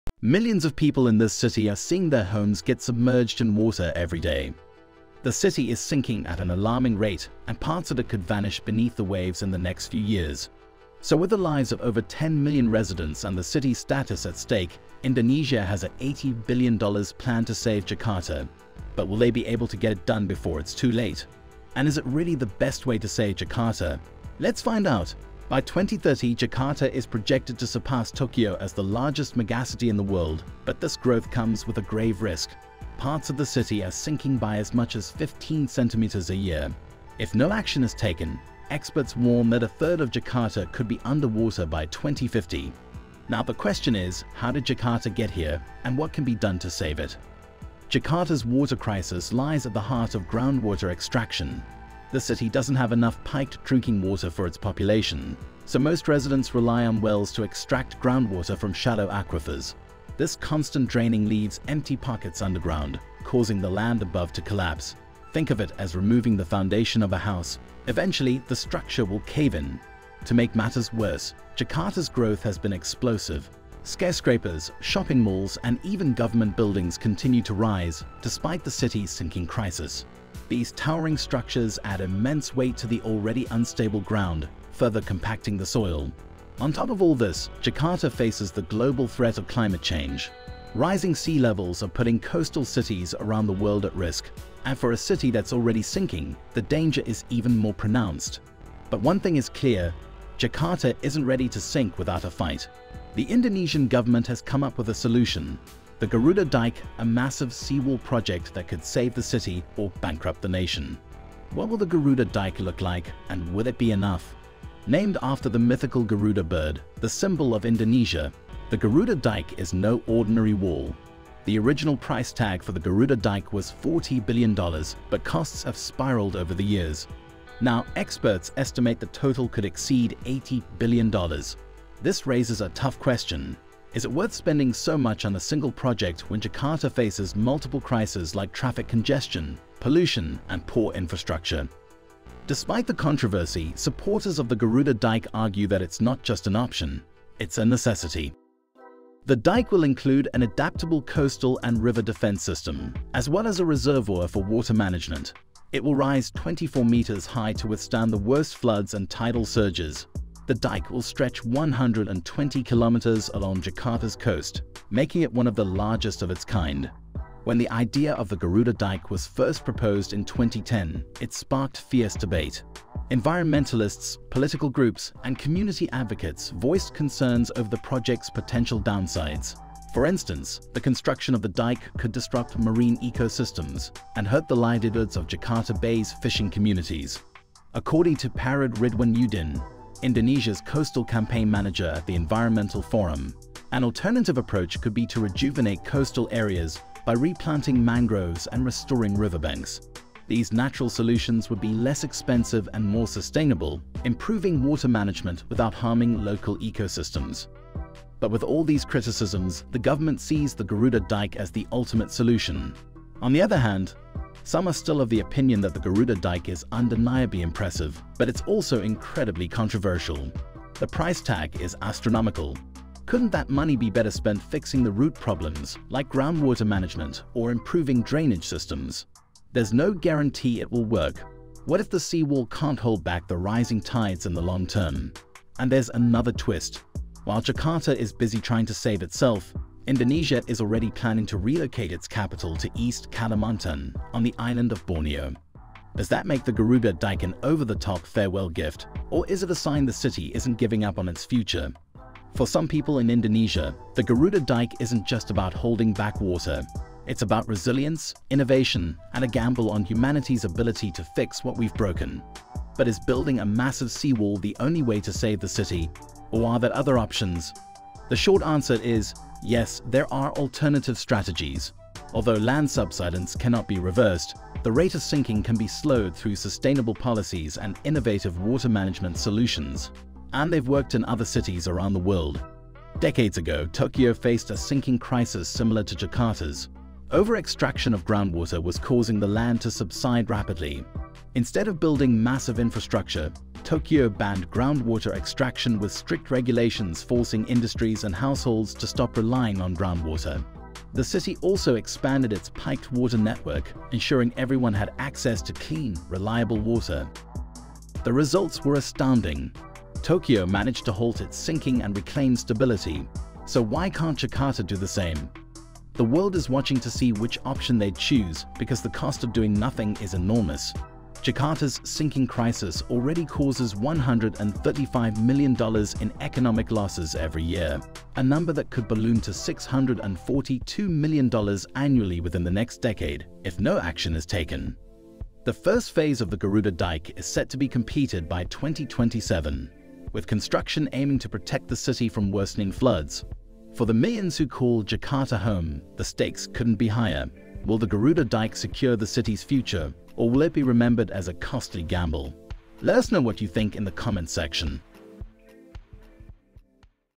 Indonesia’s $80B Mega Dam to Save Jakarta
Jakarta, one of the fastest-sinking cities in the world, is facing an urgent crisis—and Indonesia has a bold $80 billion solution: the Mega Dam project. In this video, we dive into the ambitious plan designed to protect the capital from rising sea levels and devastating floods. Discover how this massive engineering feat could reshape the future of Jakarta and set a new standard for climate resilience.